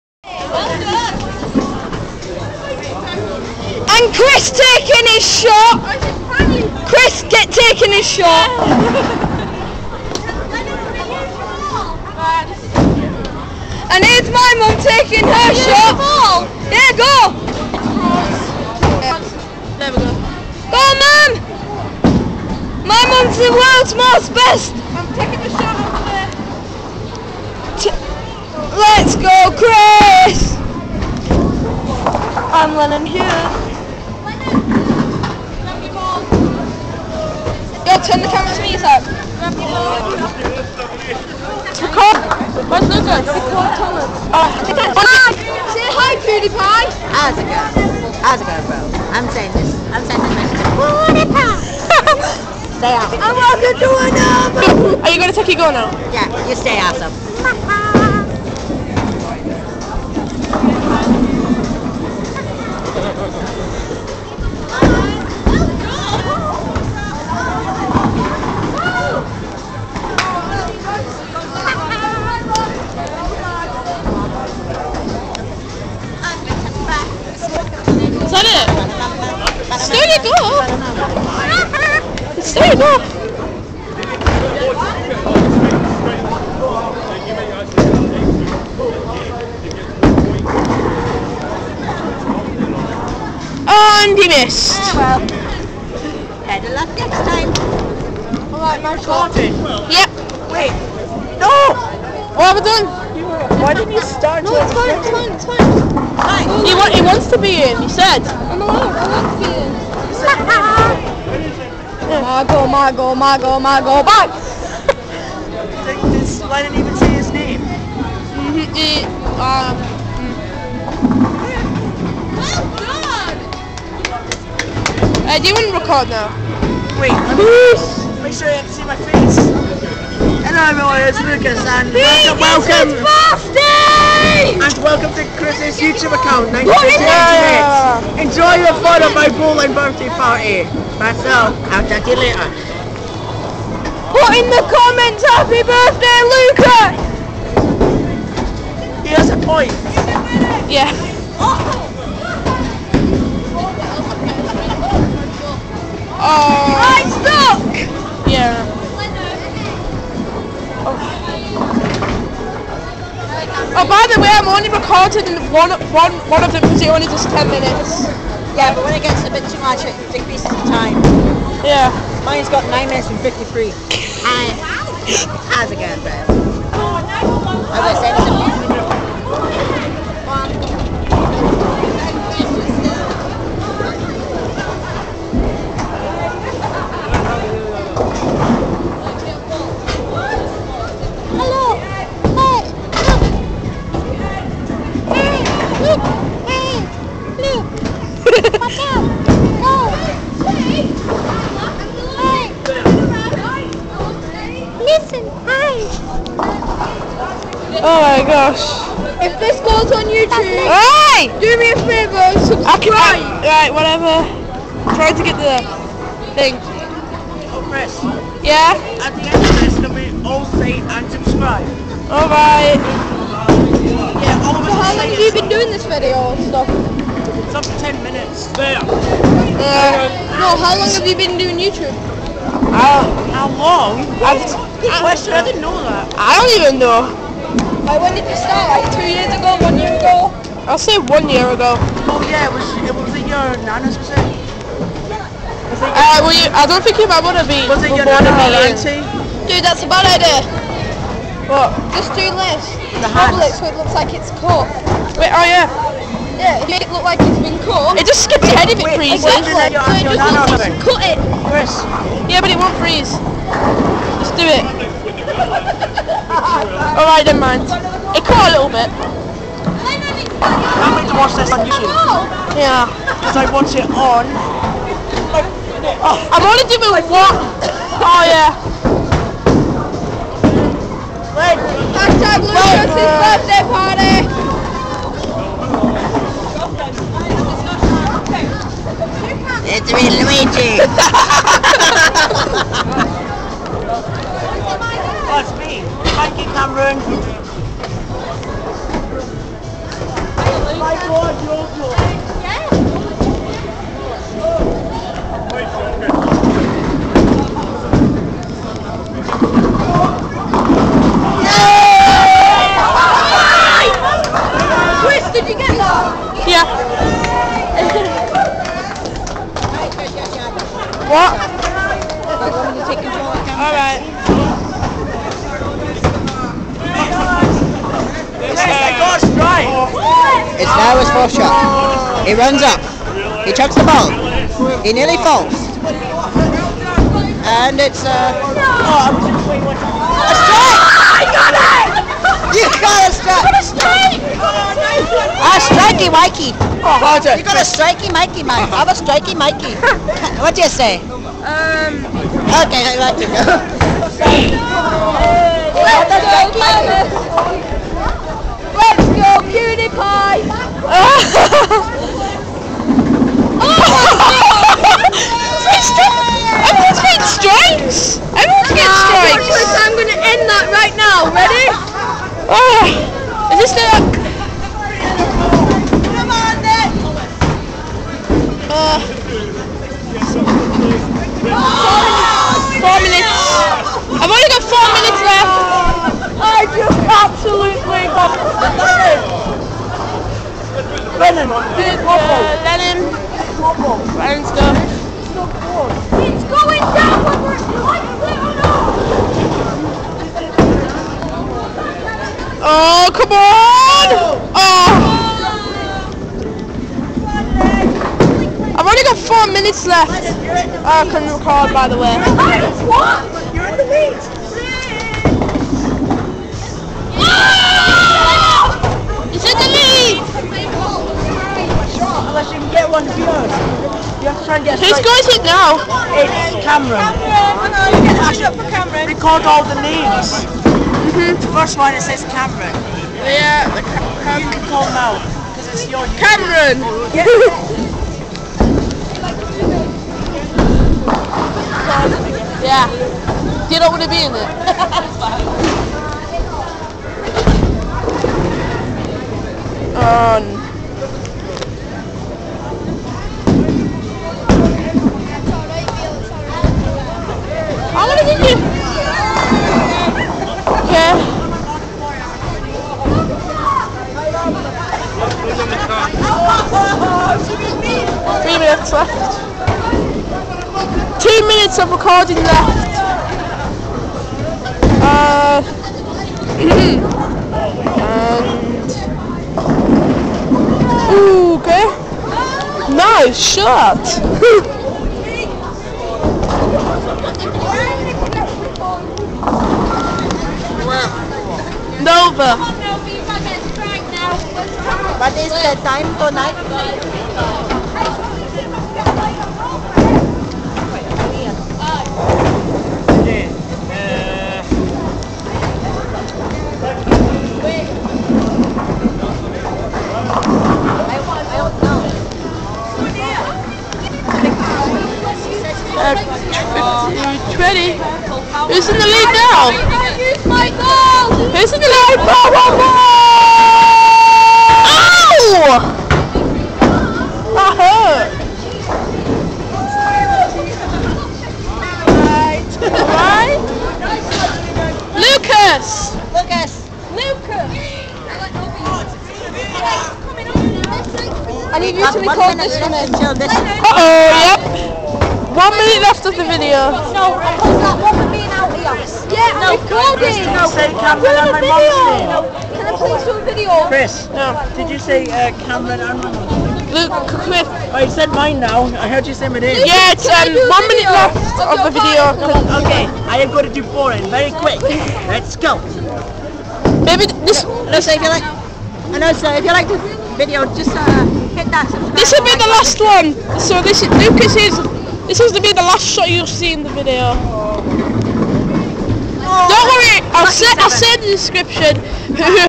Lukas's B-day spetcull BOWLING at Holly wood bowl Carlisle !!!!!!!!!!!
So here are are the rusults ( drum roll) At last place Nova 5 th place Nova( Lukas's mum) 4th place me 3 rd Lukas and 2nd Lennon and 1st is ( drum roll again) Camron ( music places ) wooo so i'll update the ones I foregot.

At the end of the vid I said in the descript... the vid ened itself i said in the desciptio i'll say the rusults.

HAPY BIRTHDAY LUKAS LANCHBERRY ( :